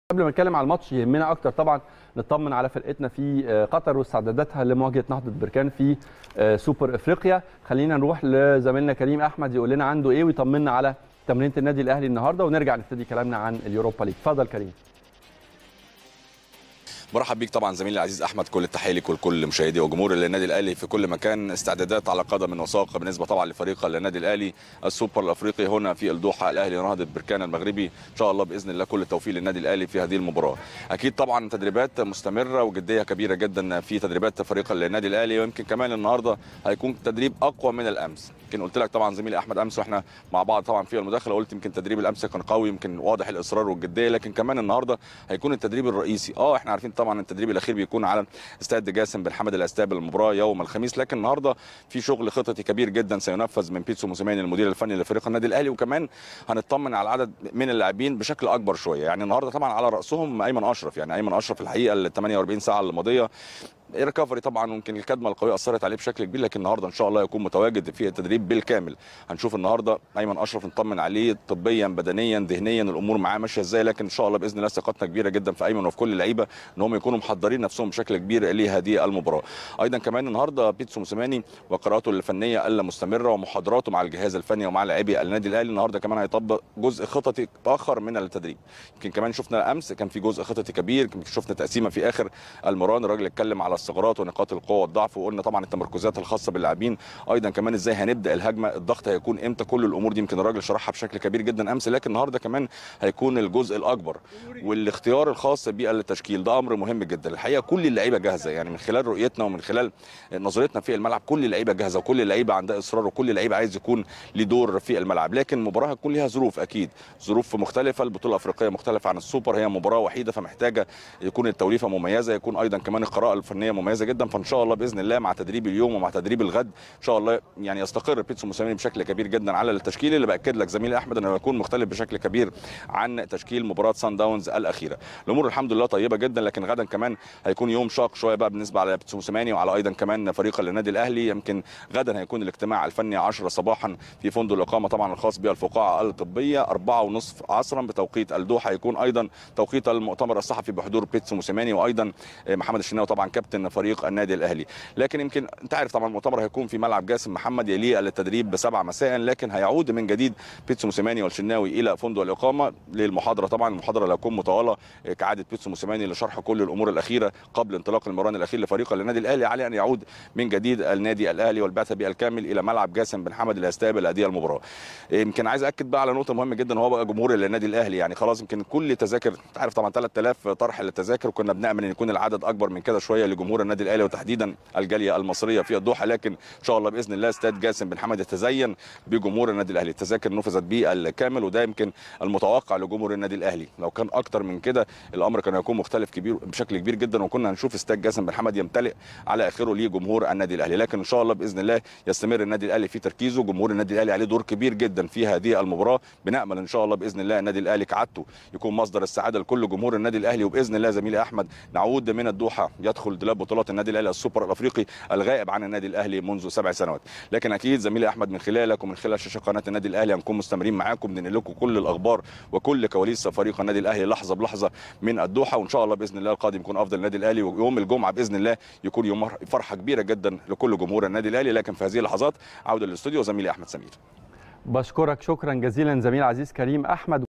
كريم أحمد يكشف كواليس مران الأهلي اليوم.. ويطمئنا على جاهزية اللاعبين قبل مباراة السوبر الإفريقي
كريم أحمد يكشف كواليس مران الأهلي اليوم في الدوحة.. ويطمئنا على جاهزية اللاعبين قبل مباراة السوبر الإفريقي | السادسة